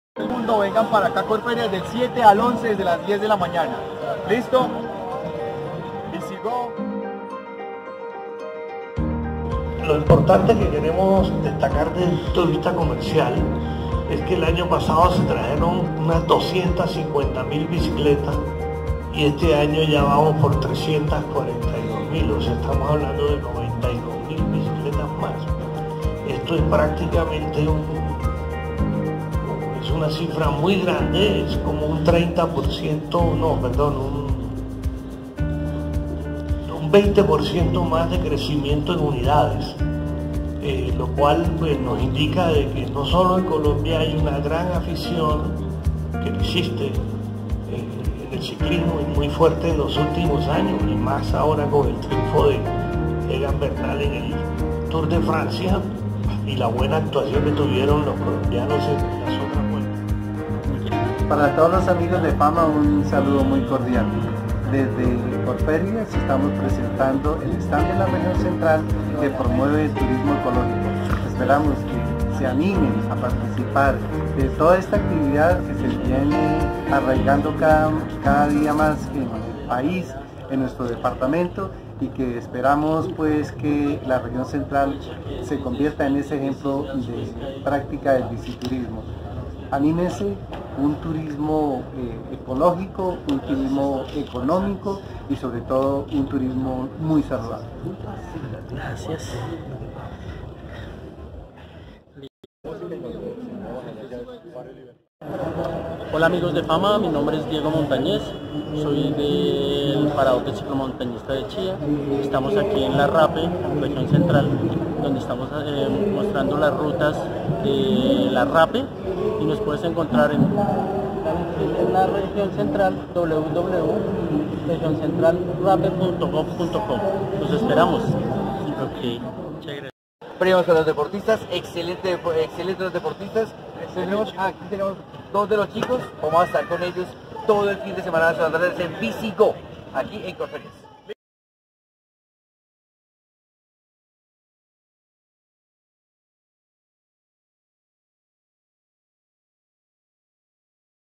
Bicigo llega rodando a Corferias
La segunda versión de esta feria de las ciclas y todo sus accesorios llegan al gran salón de Corferias. QuedenseEnCasa EchePa´LaCasa
Hola amigos de CanalFama Espero que te haya gustado el vídeo y apoya mis redes, abajo te dejo mis links.